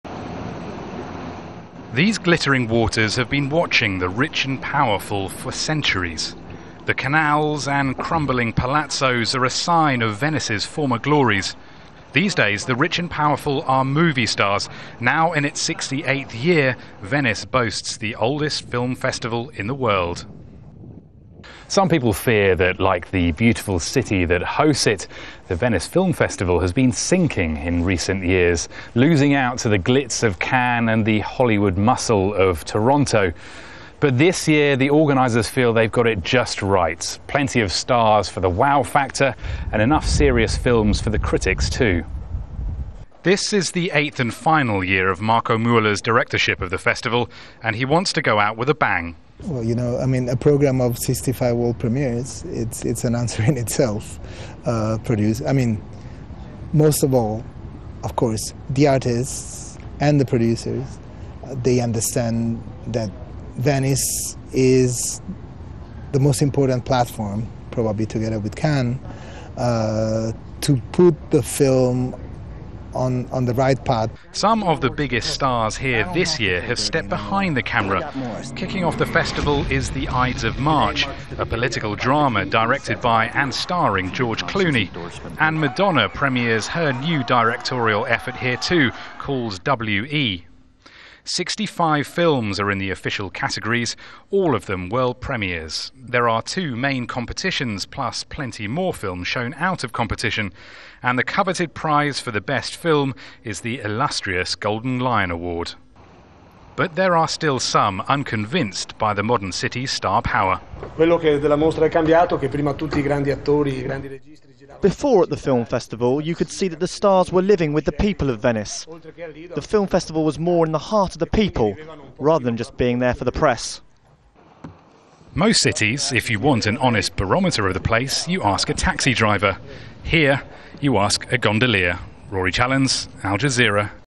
Venice to dazzle film-goers
In it's 68th year, Venice boasts the oldest film festival in the world.

Yet, there are fears that the Venice Film Festival is sinking, as other film festivals are deemed to be glitzier, more spectacular, more 'Hollywood'.

For Marco Mueller, the director of the Venice Festival for the last eight years, this year is going to outshine all of the others.

"Some of the biggest stars here this year have stepped behind the camera."

65 films are in the official categories this year, including films directed by George Clooney, and Madonna.

Al Jazeera's Rory Challands reports from Venice.